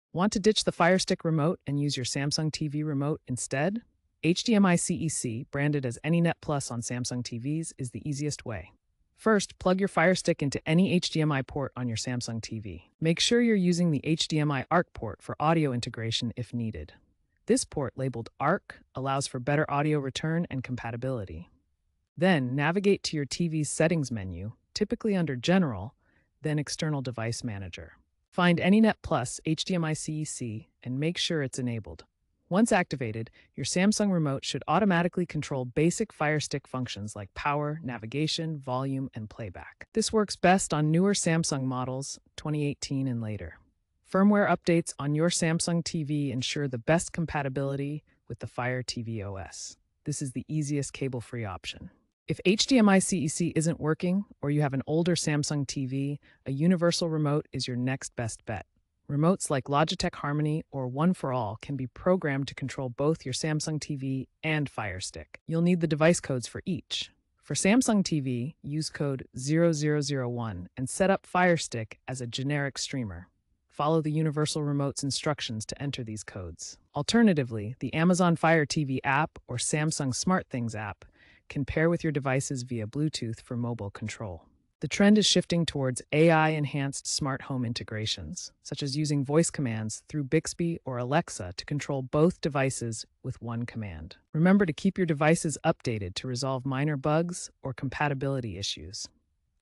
How to Use a Samsung TV Remote with Firestick – HDMI-CEC & Universal Remote Setup
Control your Firestick with a Samsung TV remote using HDMI-CEC. Learn setup steps, enable power/volume control, and optimize your universal remote for smooth streaming experience.